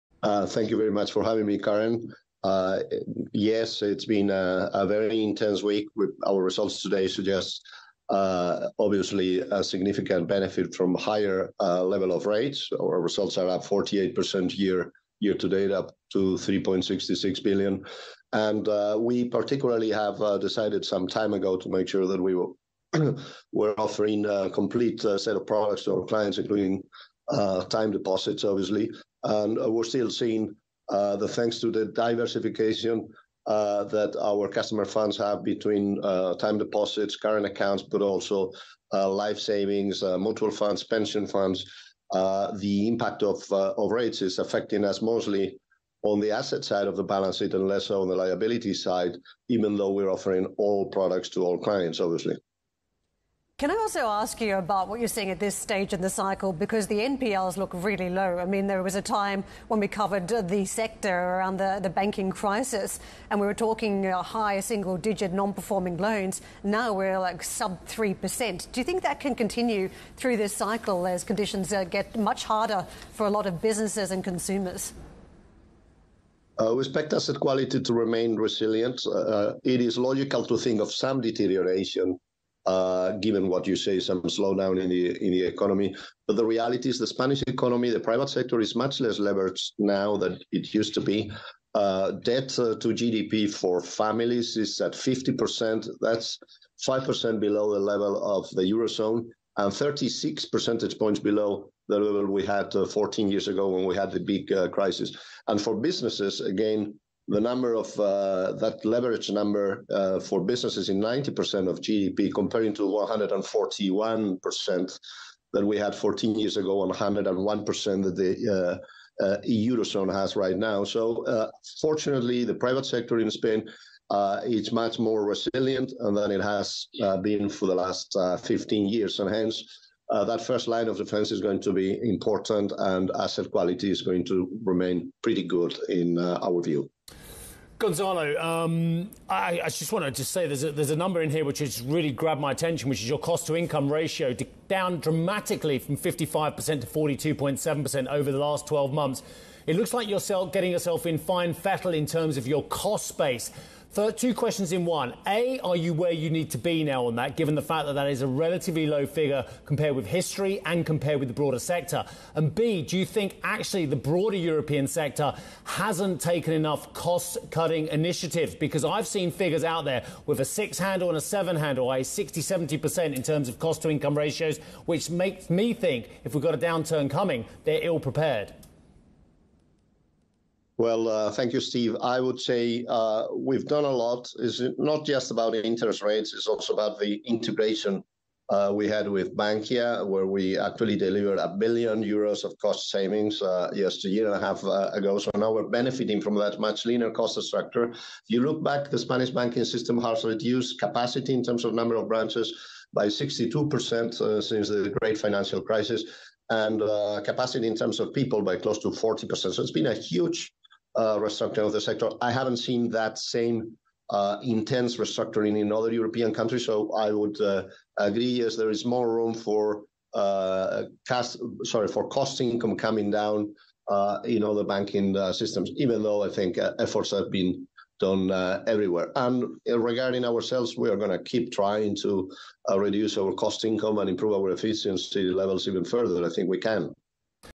CaixaBank CEO: Higher rates brought 'significant benefit' to our results
Gonzalo Gortazar, CEO CaixaBank, discusses third-quarter earnings.